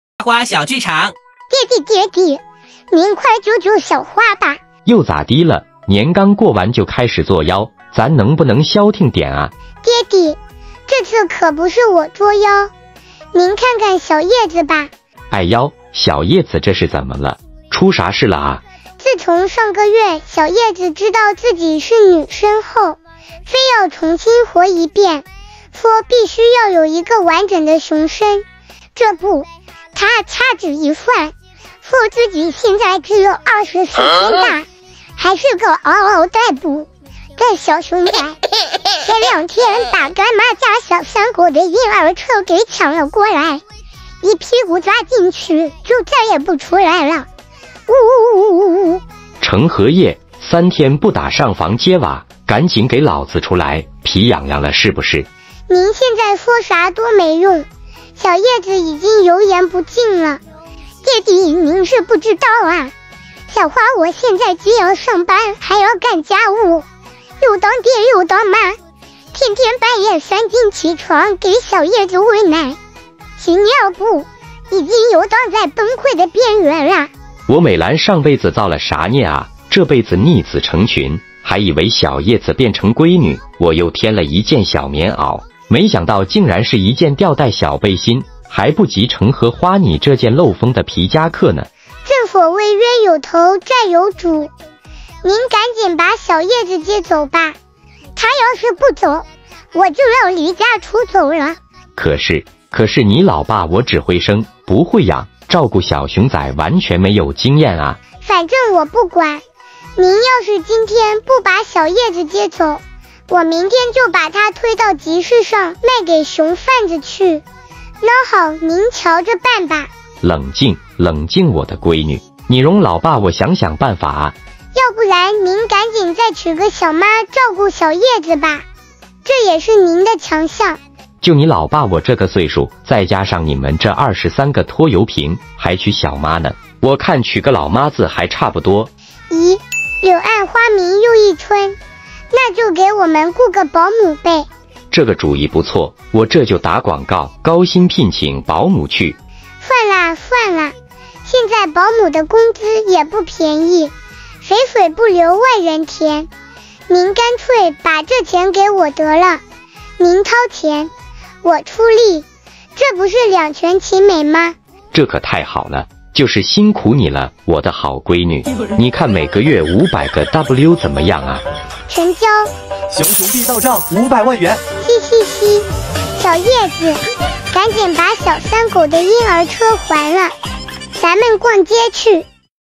《花花小剧场》年过完了，花叶姐妹又开始在爸爸面前作yao啦，嘻嘻嘻# 来这吸熊猫
🙏感谢Youtube 感谢关注
🙏每日更新 花叶视频
🙏坚持原创 守护国宝
🙏视频剧场 纯属虚构